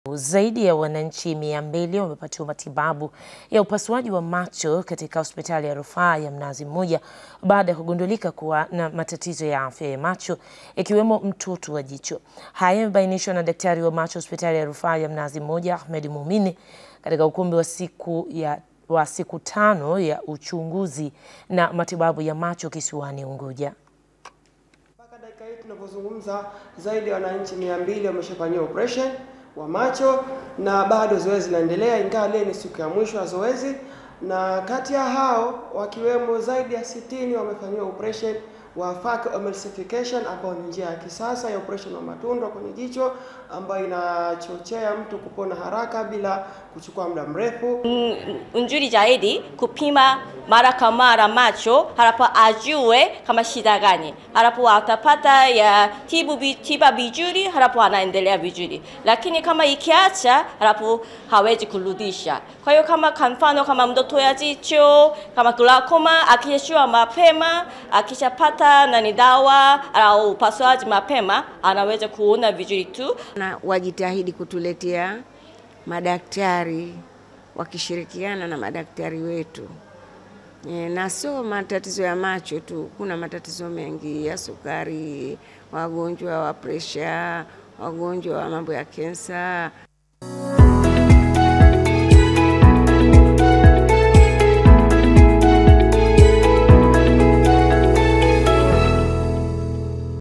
Zaidi ya wananchi 200 wafanyiwa upasuaji wa macho Zanzibar
Zaidi ya wananchi 200 wamepatiwa matibabu ya upasuaji wa macho katika Hospitali ya Rufaa ya Mnazimmoja – Zanzibar baada ya kugundulika kuwa na matatizo ya afya ya macho ikiwemo mtoto wa jicho.

Wahariri | John Mbalamwezi